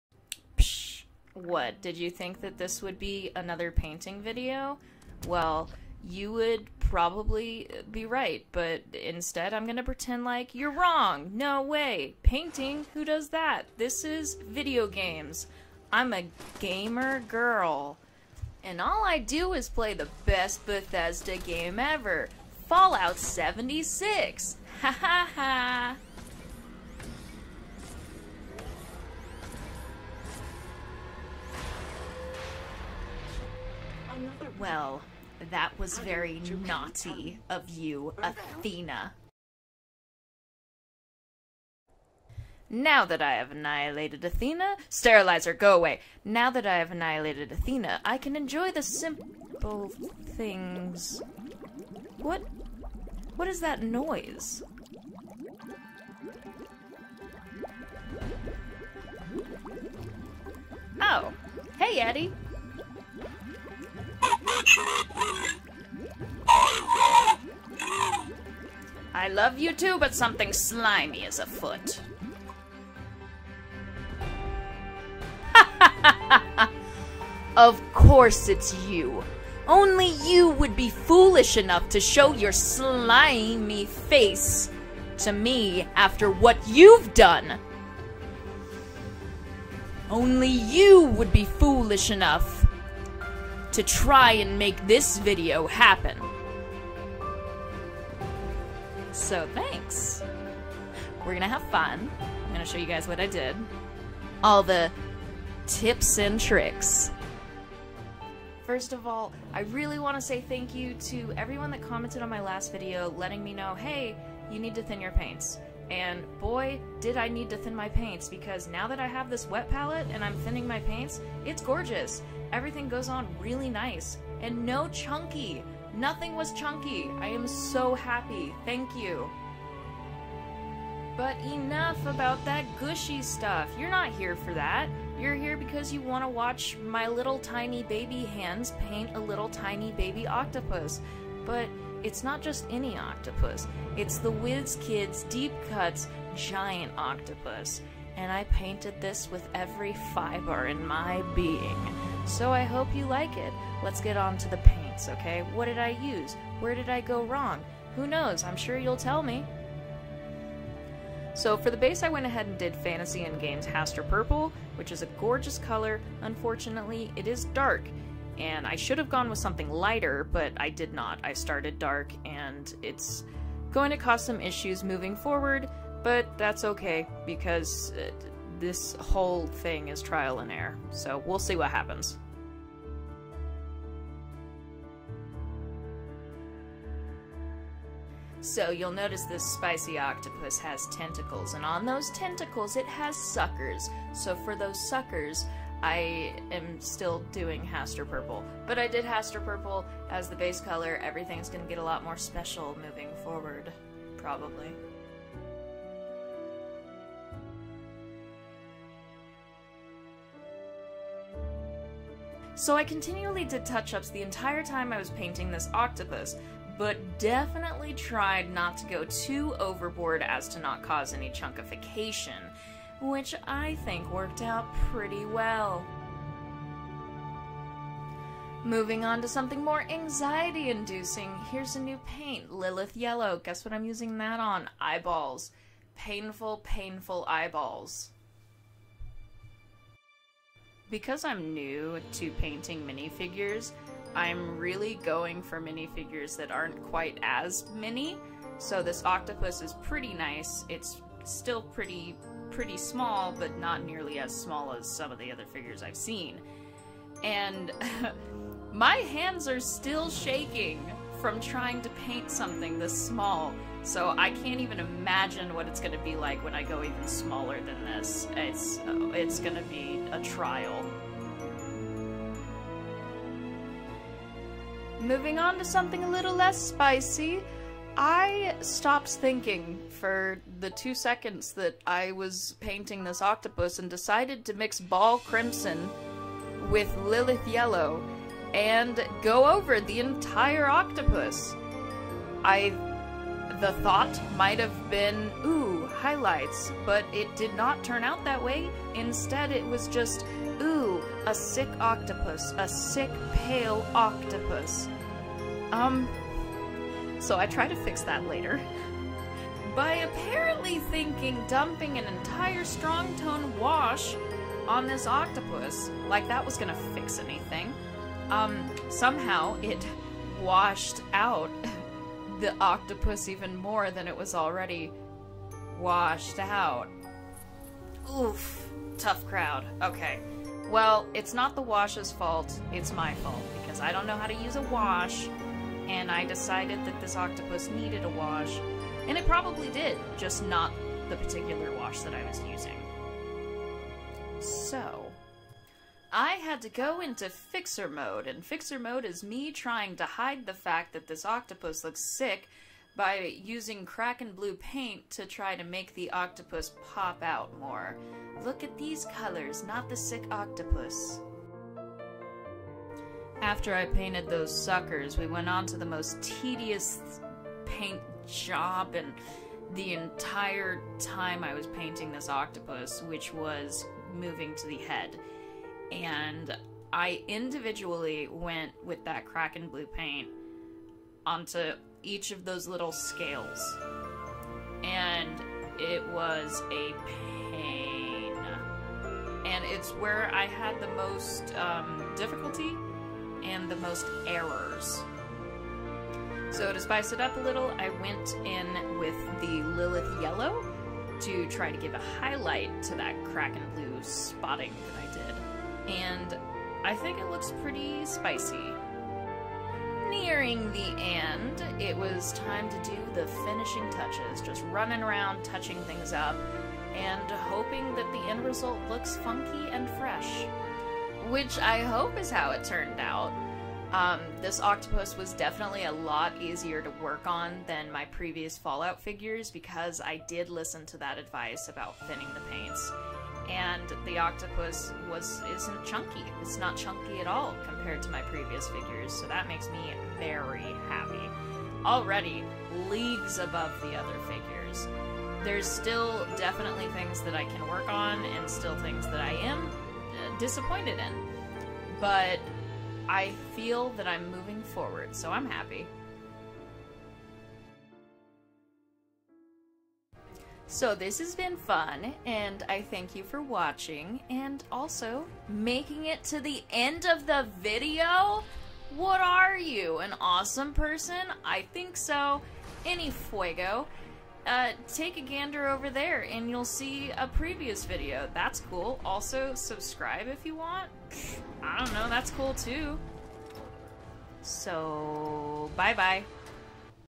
WizKids Deep Cuts Giant Octopus Mini PAINTED
Hey All, Hope you've enjoyed my first painting video! This little Octopus was fun to work on and I'm happy with the end result. As always, If you have any advice for me then please let me know down in the comments how I can improve.

Paint Army Painter color
Army Painter Wash